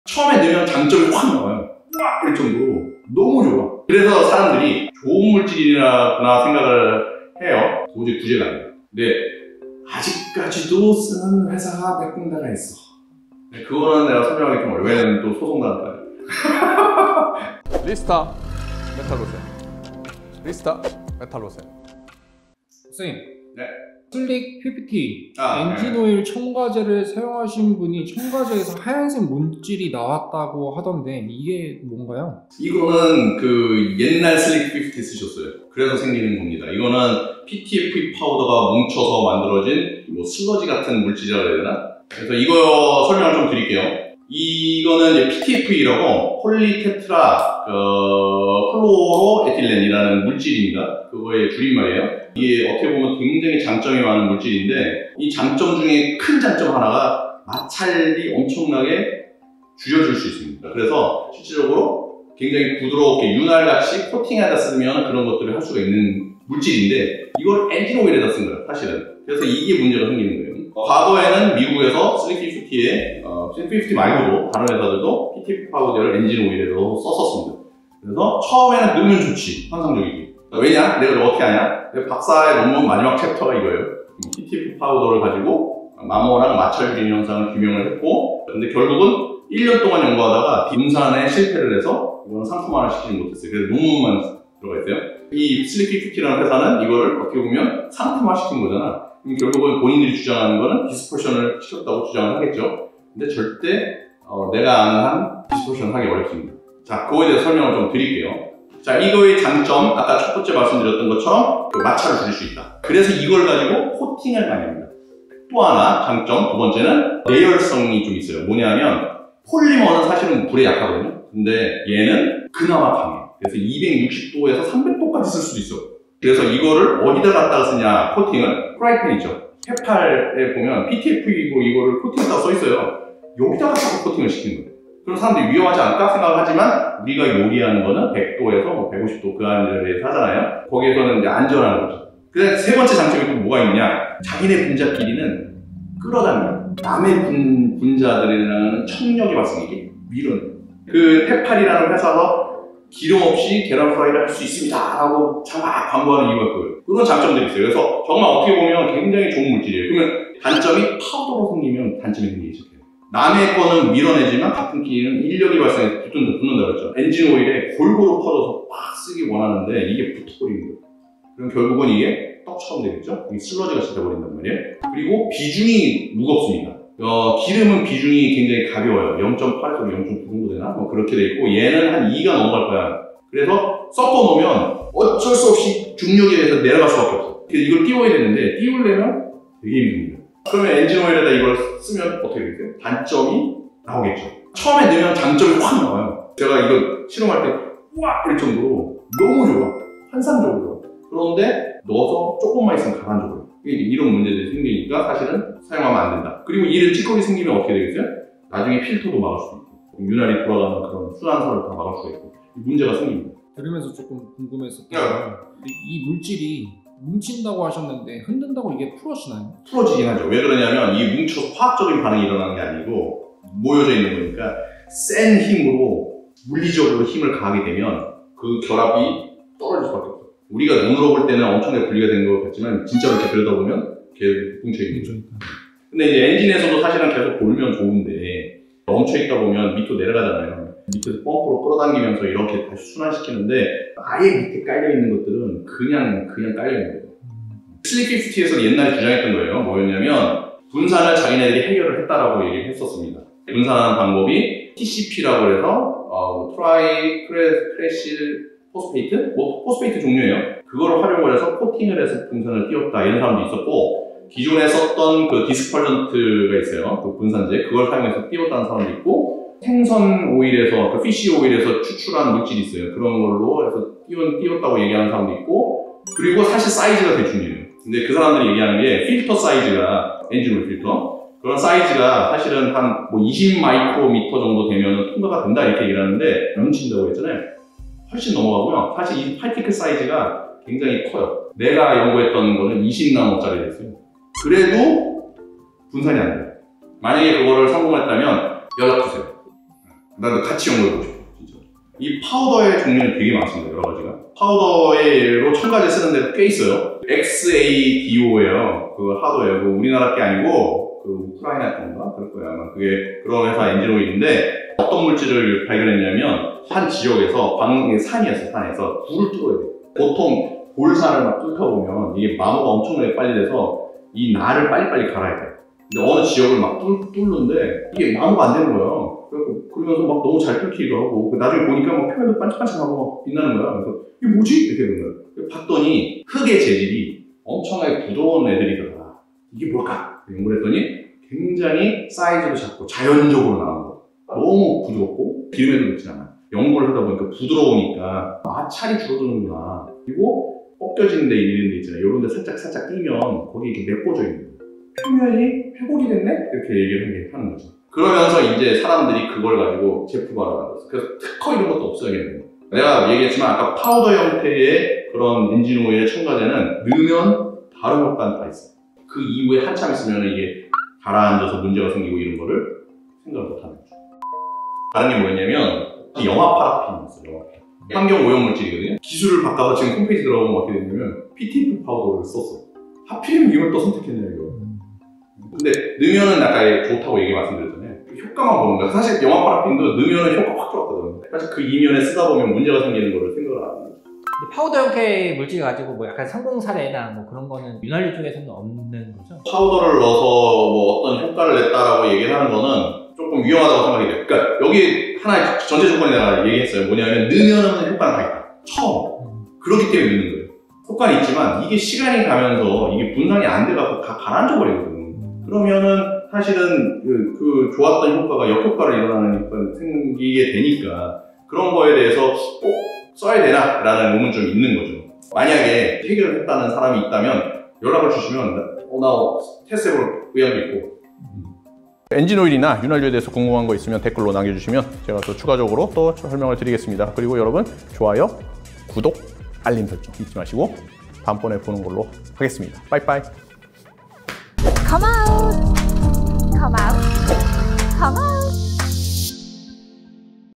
EP.117 환상적이지만 위험하다? OO 엔진오일 첨가제의 두 얼굴!
OO이 들어간 엔진오일 첨가제, 초기엔 환상적이지만 사용은 안전할까요?
엔진오일 첨가제도 꼭 알고 사용해야 합니다!

Timeline:
0:00 인트로!
0:22 첨가제에서 나온 하얀 물질?
1:51 테프론의 장점!
2:44 단점은?
3:10 테프론을 엔진오일에 쓰면?
3:37 뭉치는 테프론, 흔들면 풀어질까?
5:04 첨가제에 쓰이는 파우더!

✔진짜 전문가가 만든 "엔진오일 첨가제", 리스타 S1

✔ 혁신적인 프리미엄 롱라이프 엔진오일 "리스타 메탈로센"